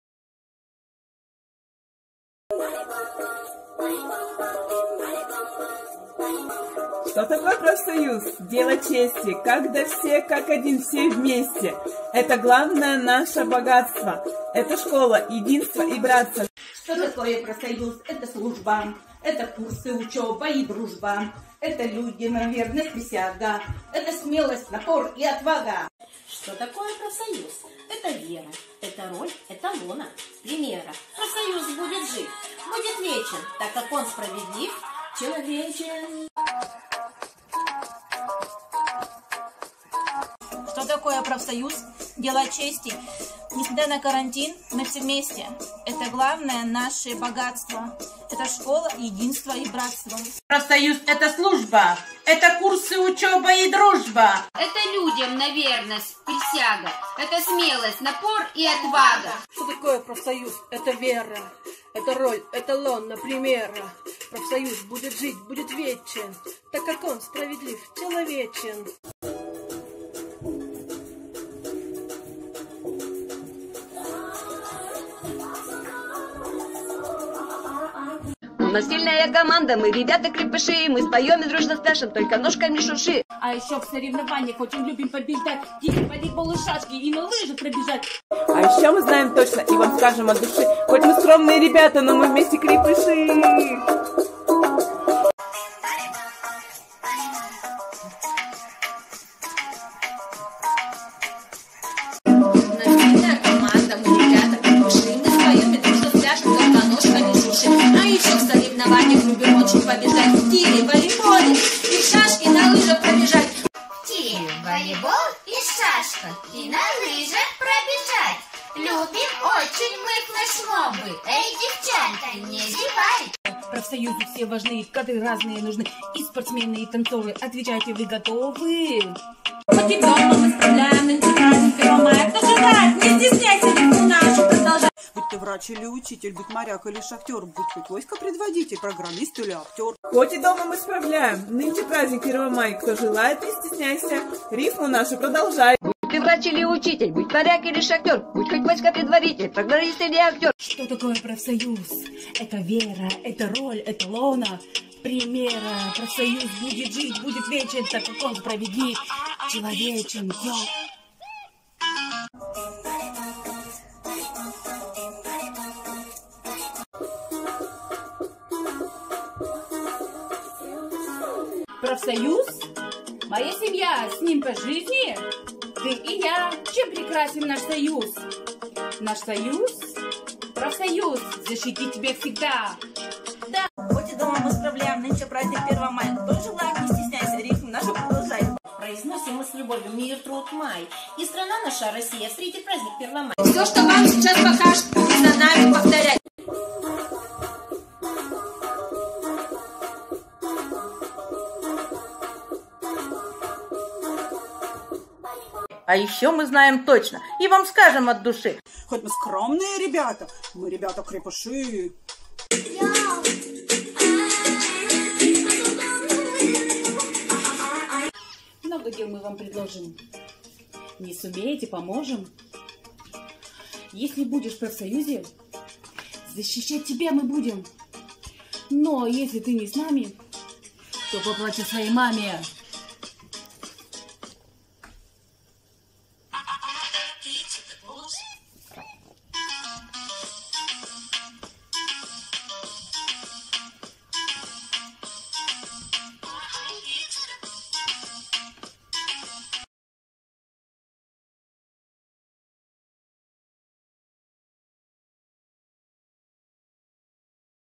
Воспитатели детсада написали рэп про Первомай
Представители профсоюзов призывают оставаться дома. Правда, многие фото сделаны на рабочих местах и на улице. Главный лозунг праздника «Солидарность сильнее заразы».

Некоторые коллективы не останавливаются на фотопоздравлениях. Так, коллектив детского сада «Крепыш» из Режа снял клип, в котором няни, воспитатели, музыкальный руководитель исполняют песню собственного сочинения в стиле рэп.